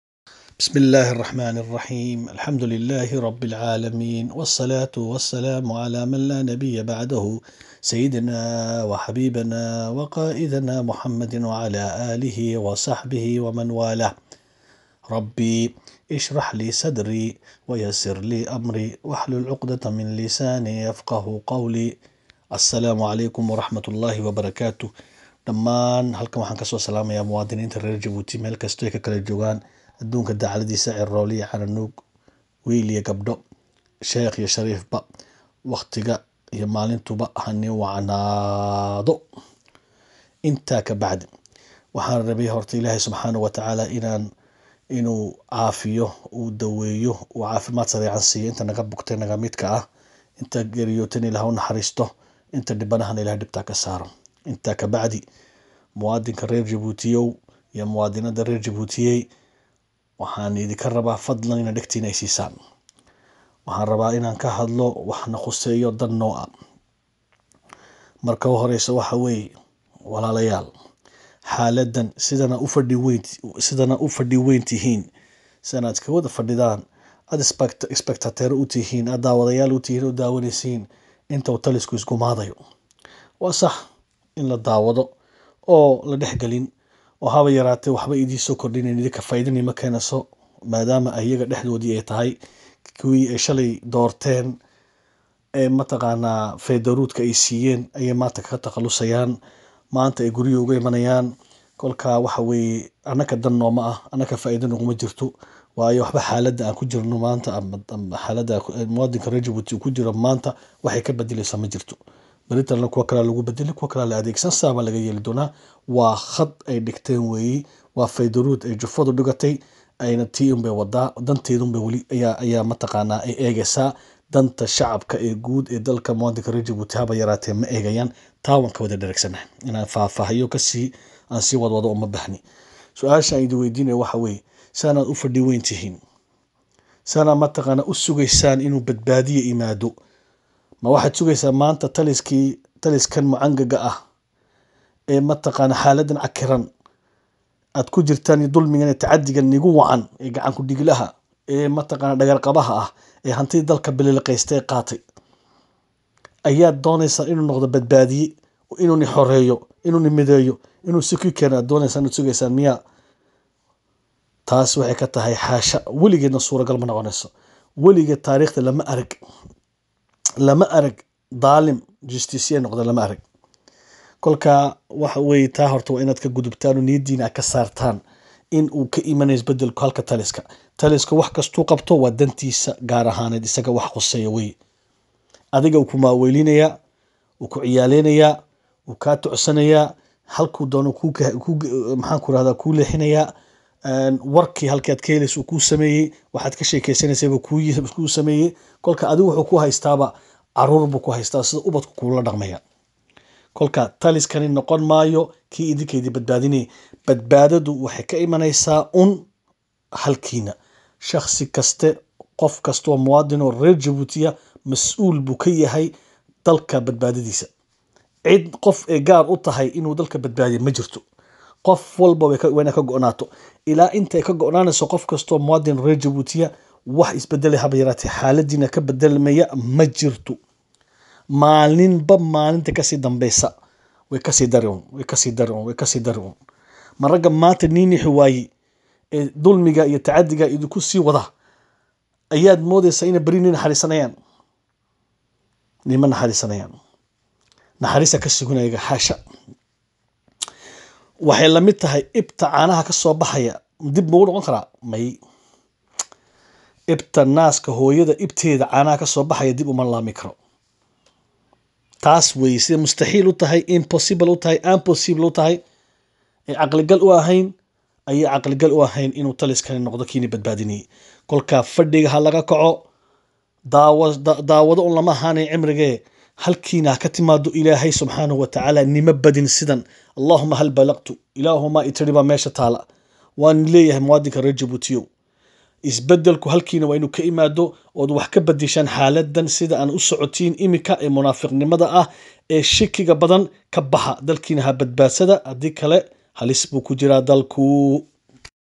🔴🚨🇩🇯🇸🇴🇪🇹DJIBOUTI💡DHIIRIGELIN💡👂☝📢
FARIIN KU SOCOTA MUWAADINKA REER JABUUTI EE DULMIGA IYO TACADIGA LAGU HAAYO.

CE MESSAGE EST DESTINÉ À MES COMPATRIOTES DJIBOUTIENS ET DJIBOUTIENNES QUI VIVENT DANS UNE SITUATION PRÉCAIRE.

DHAGEYSI WACAN...